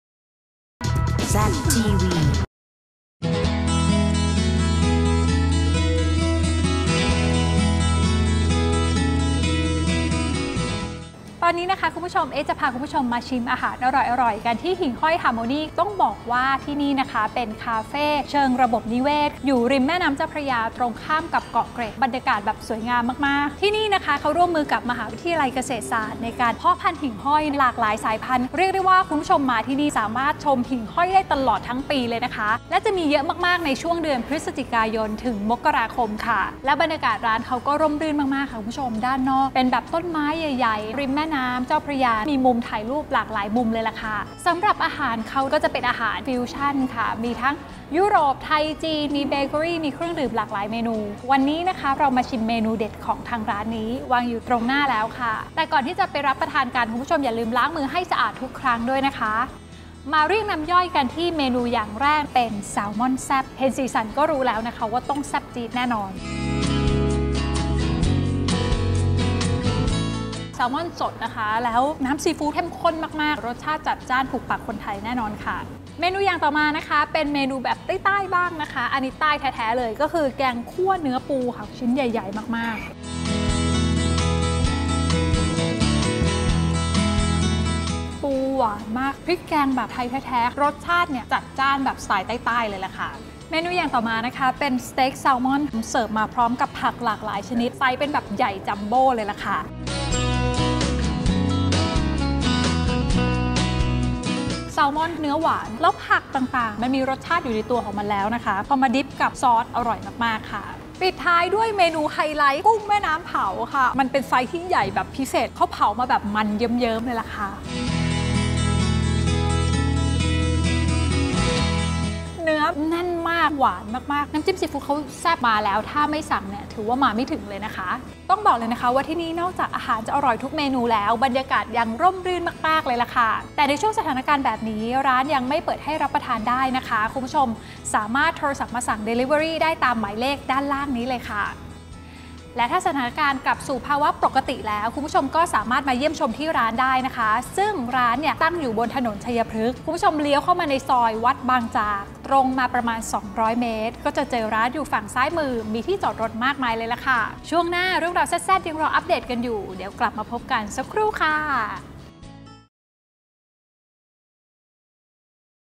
Z Café ชวนกินอาหารเมนูหลากหลาย บรรยากาศในสวน ชมหิ่งห้อยระยิบระยับ ที่ หิ่งห้อย ฮาร์โมนี่ ถนนชัยพฤกษ์
“เอ๊ะ อิศริยา”  ชวนกินอาหารเมนูหลากหลาย บรรยากาศในสวน  ริมแม่น้ำเจ้าพระยา  ชมหิ่งห้อยระยิบระยับ ที่ หิ่งห้อย ฮาร์โมนี่  ถนนชัยพฤกษ์ ซอยวัดบางจาก
เมนูแนะนำ : แซลมอลแซบ , แกงคั่วเนื้อปู และกุ้งแม่น้ำเผา

Instargram : ztvthailand